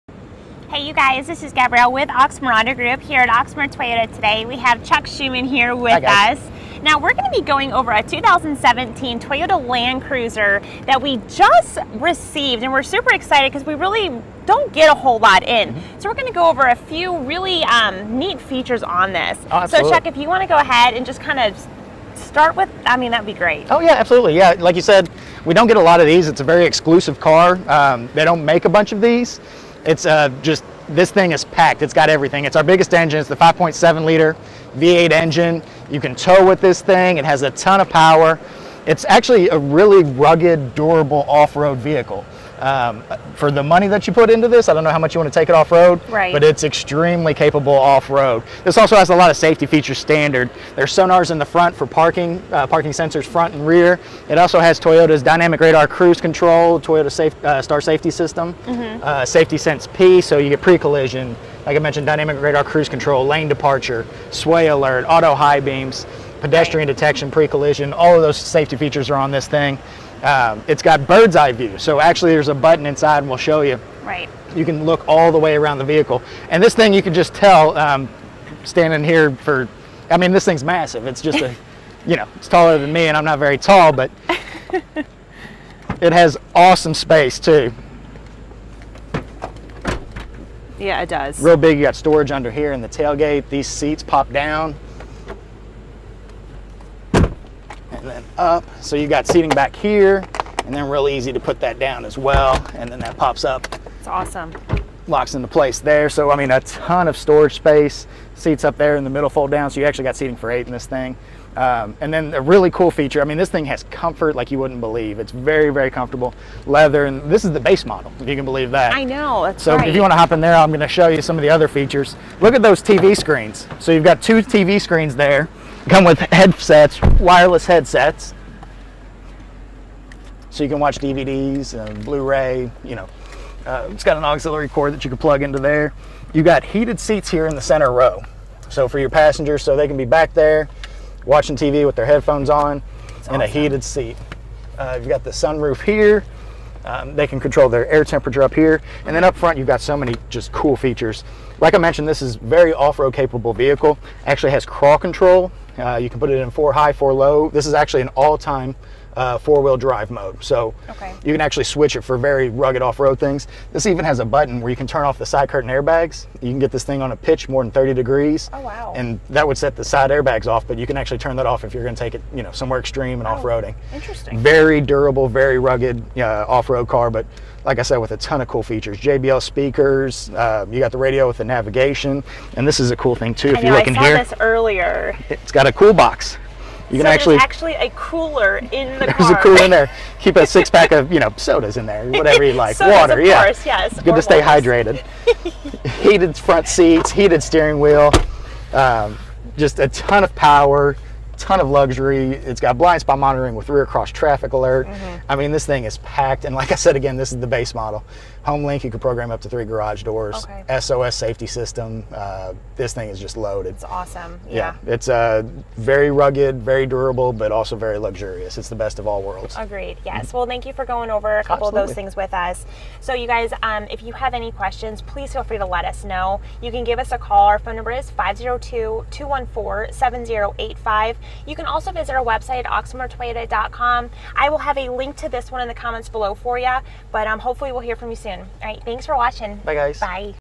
2017 Oxmoor Toyota Land Cruiser for sale in Louisville, Lexington, Elizabethtown KY
The 2-17 Toyota Land Cruiser for sale at Oxmoor Toyota in Louisville, Lexington, Elizabethtown KY.  Video Walk Around.

Oxmoor Toyota
8003 Shelbyville Road